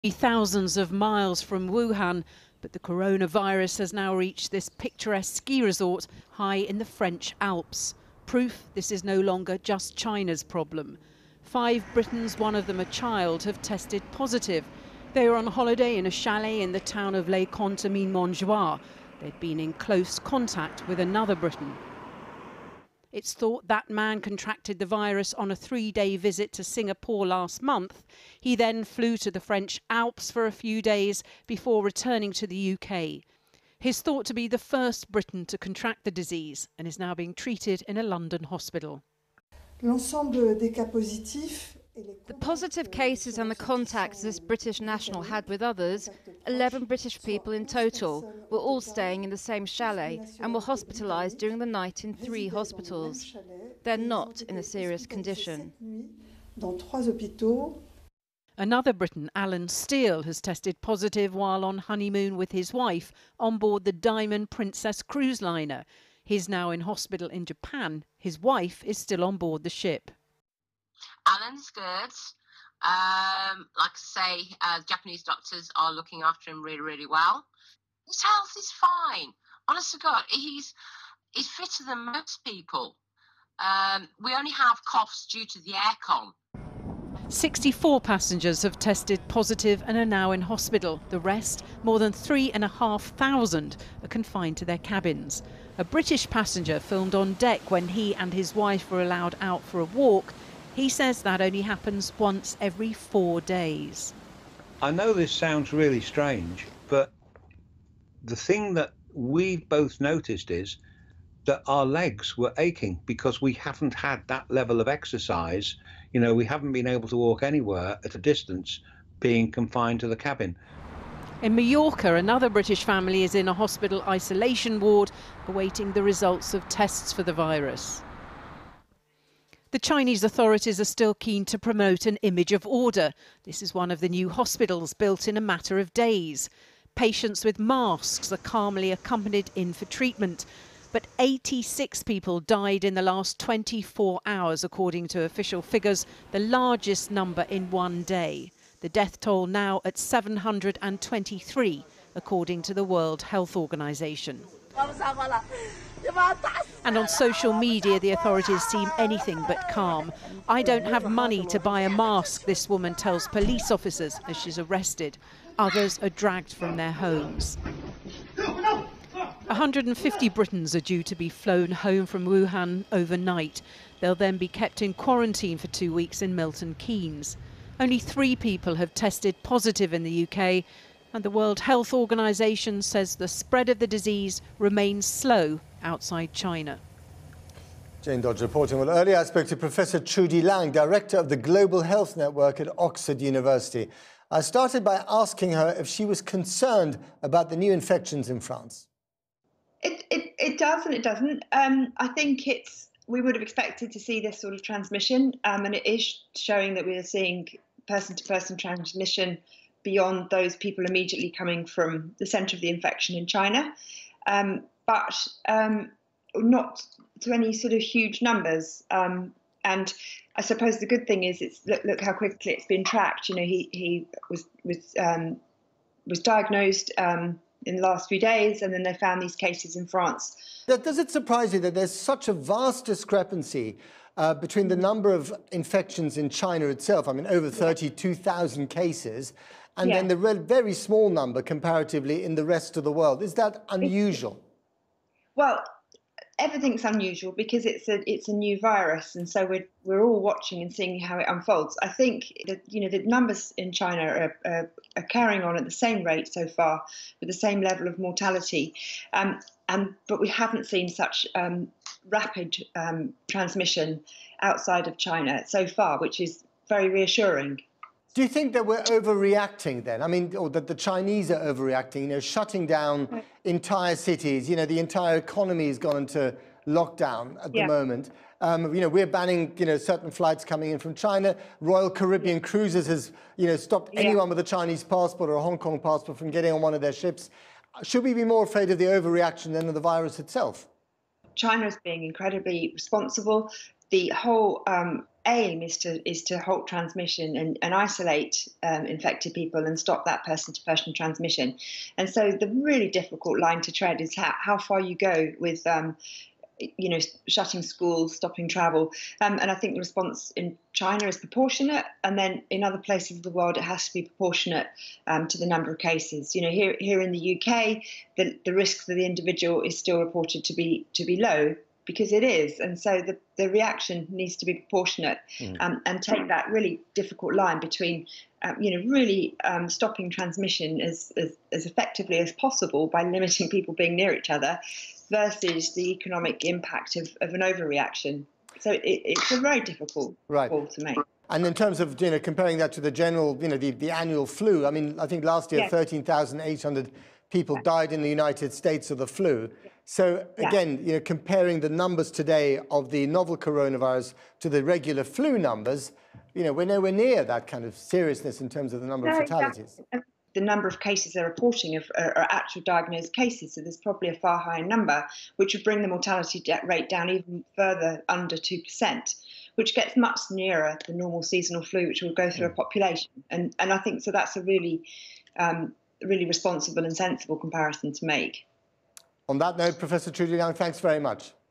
Coronavirus: Five Brits test positive for virus in French ski chalet
The five Britons who've tested positive for coronavirus at a ski chalet in the Alps had been nowhere near China. Nor had the man who they got it from.

In a worrying new development - French officials said the four adults and a 9 year old child had been in contact with another British citizen who'd recently returned from Singapore - and was later found to have coronavirus after he had returned to his home in Brighton.